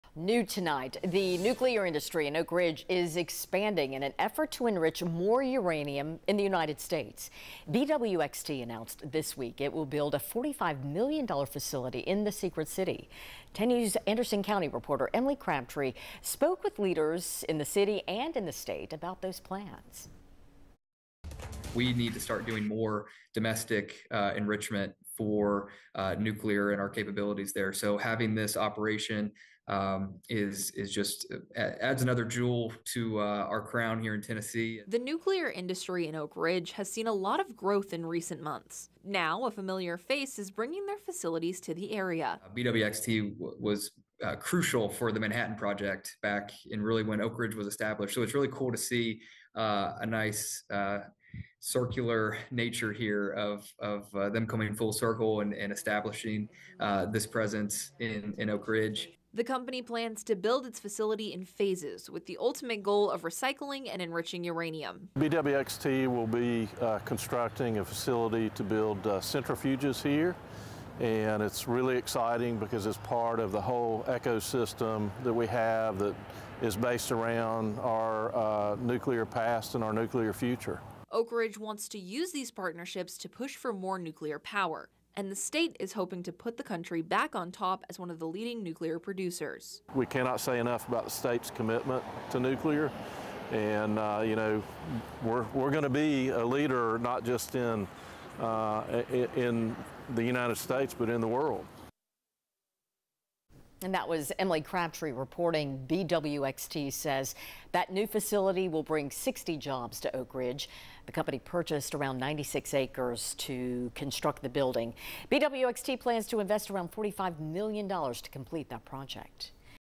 BWXT announces new nuclear facility in Oak Ridge
The company said the new facility will bring 60 jobs to the city.